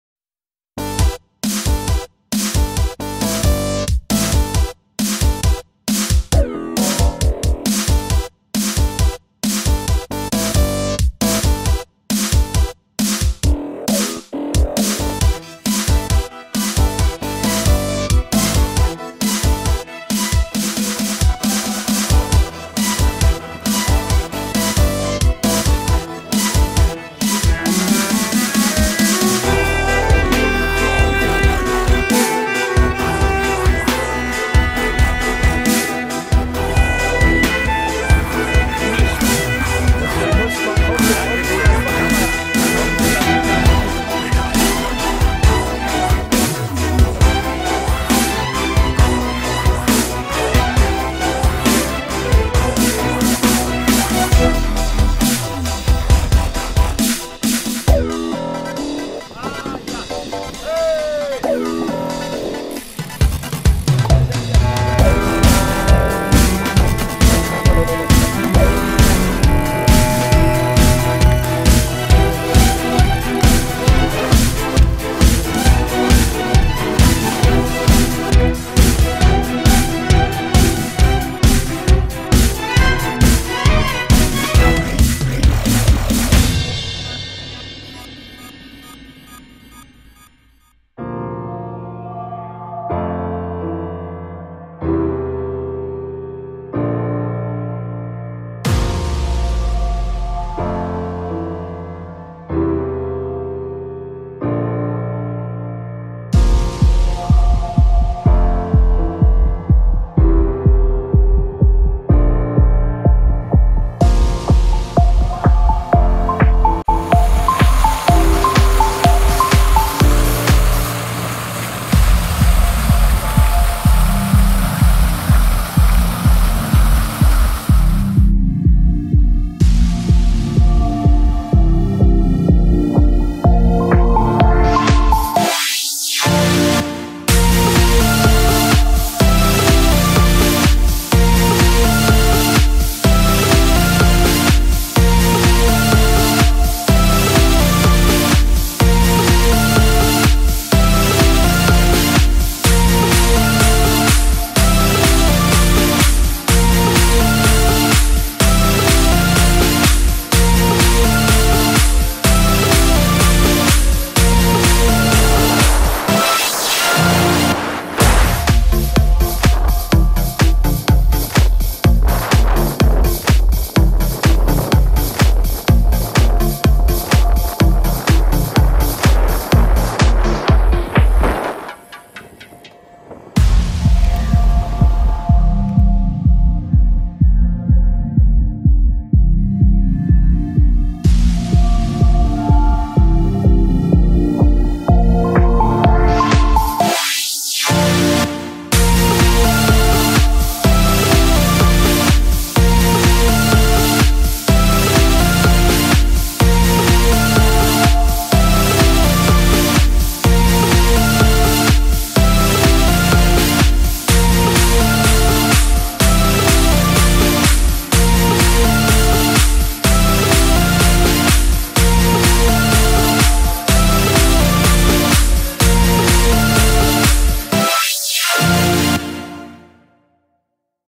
2. Etappe Rennradtour Berlin-Luxemburg am 10.06.2018
Nicht ganz so heiß wie am ersten Tag, jedoch mehr Auf und Ab: 1.285 hm. Auch die 157 km Strecke und ein Durchschnit von 20,6 km/h sorgten teilweise für kräftige Atmung. Und die Versorgung klappte prima...